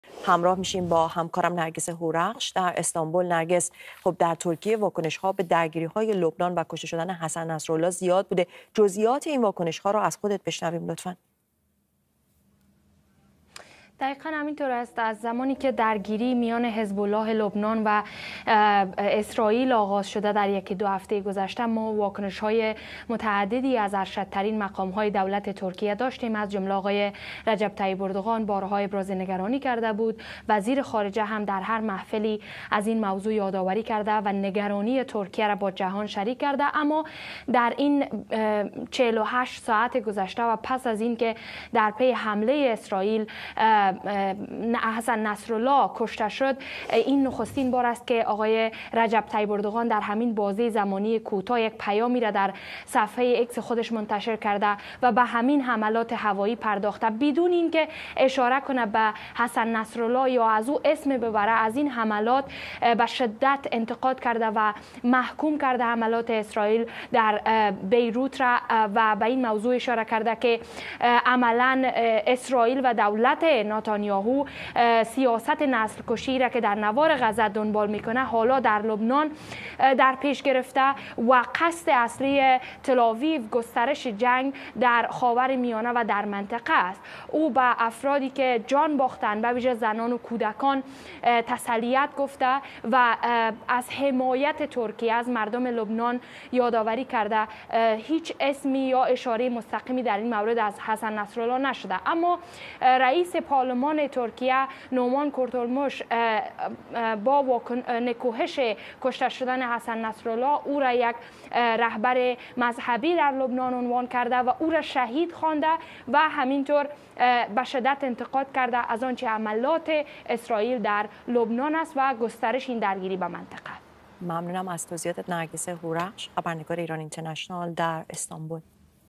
واکنش اردوغان به کشته‌شدن حسن نصرالله
رجب طیب اردوغان بدون اینکه از حسن نصرالله نامی ببرد، حملات اسرائیل به بیروت را به‌شدت محکوم کرد و گفت، «اسرائیل، سیاست نسل‌کشی را در لبنان دنبال می‌کند.»

نرگس هورخش، ایران اینترنشنال گزارش می‌دهد